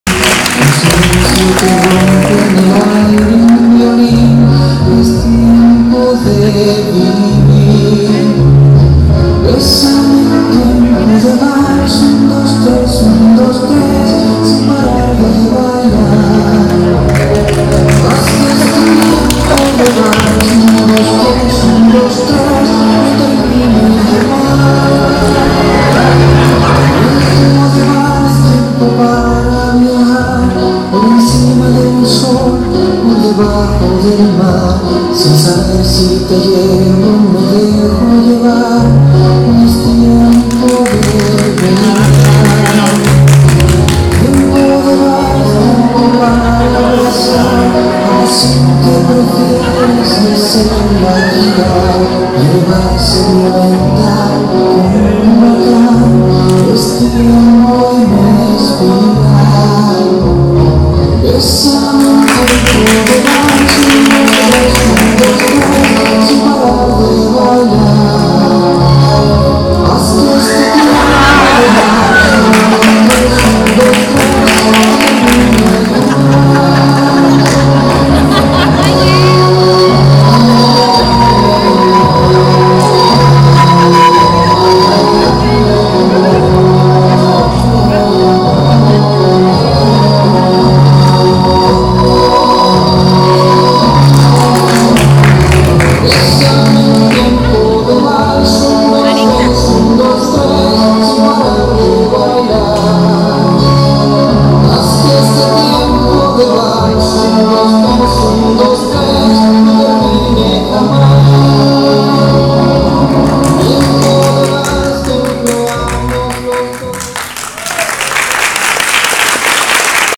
LOS ESCLAVOS MODERNOS DE FRAPOM 3-4
Solo se cumple 15 años una vez...
Obra de teatro dirigida por Guillermo Vizcaya, donde se describe la historia de una familia la cual quería aparentar un estatus que no poseían!